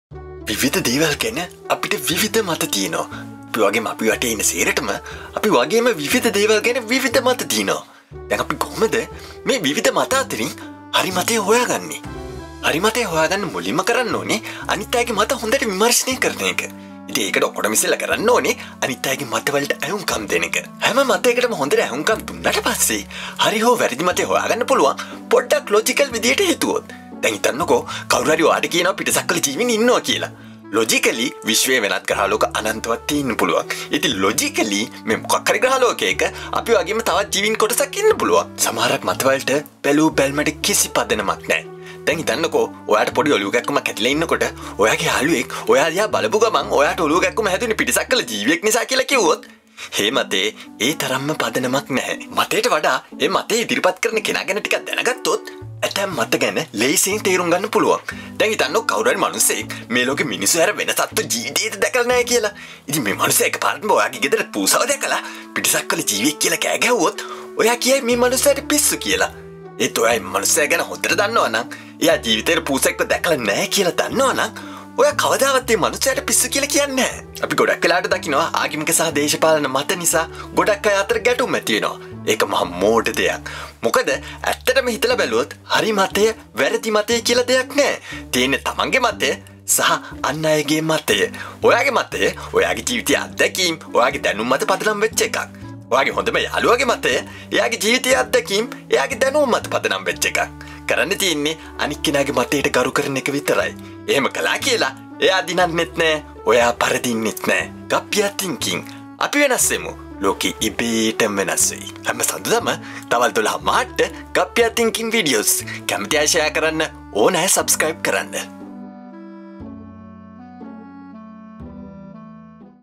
How to choose the right opinion - GappiyaThinking (Epi-17)
How do we know which opinion is the correct opinion?

12.30 PM Sri Lankan time.
5PM Winter / 6PM Summer in Sydney time.

Written Directed and Produced by Gappiya & LillyWhite.
Music Written and Produced by Gappiya.